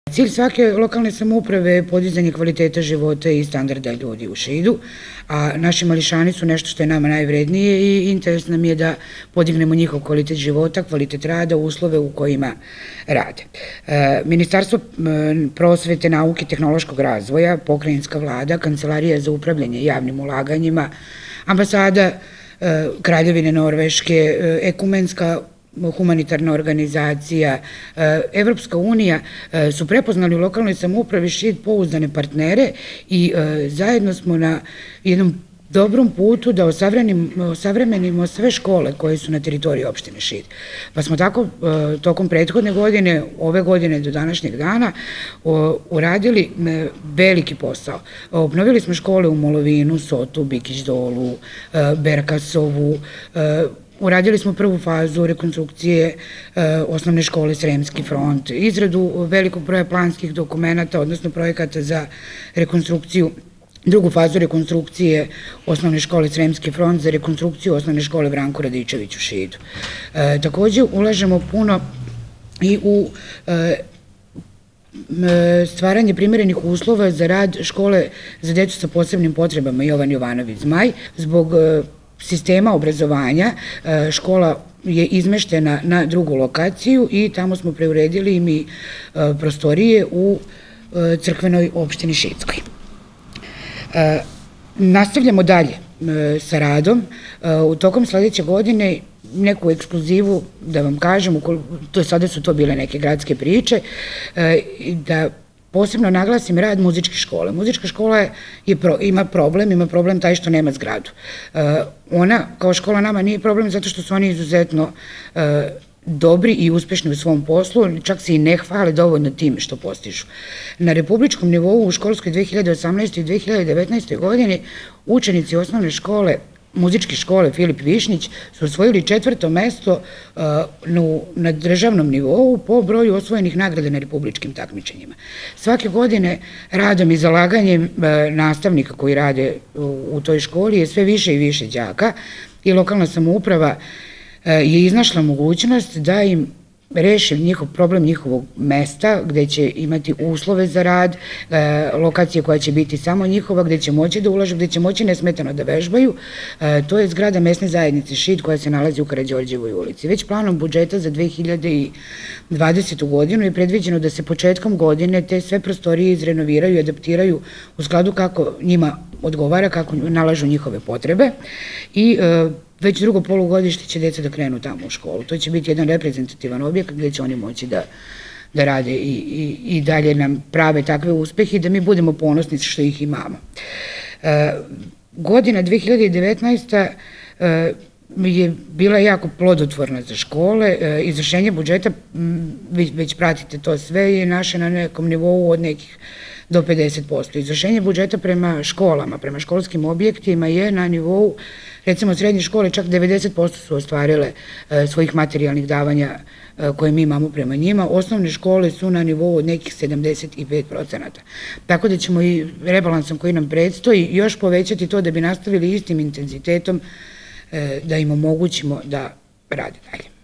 KZN - Milenka Subić - Školstvo u Šidu
Tema današnje konferencije za štampu u prostorijama SNS Šid bila su ulaganja  u predškolsko, školsko i srednjoškolsko obrazovanje. Ova godina  bila je bogata sa investicijama tog tipa u našoj opštini .